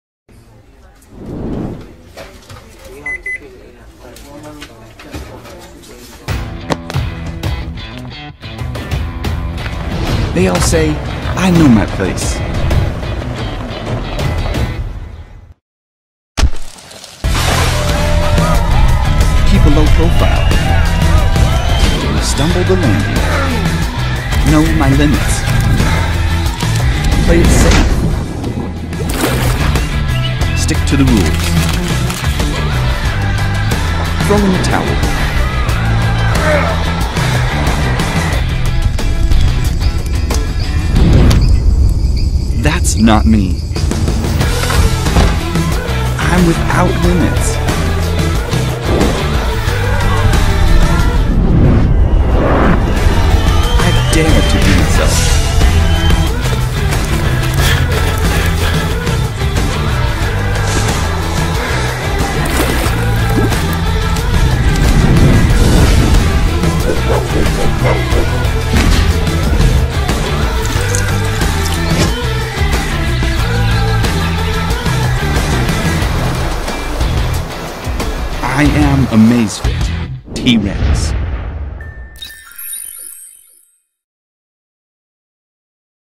Умные часы Xiaomi Amazfit T-Rex (A1919)
Умные часы Amazfit T-Rex имеют 1.3-дюймовый AMOLED-экран, с разрешением 360x360. Изделие оборудовано удобным и приятным для кожи ремешком, который изготовлен из термопластичного полиуретана. Конструкция 47.7 х 47.7 х 13.5 мм с ремешком весит всего 58 г.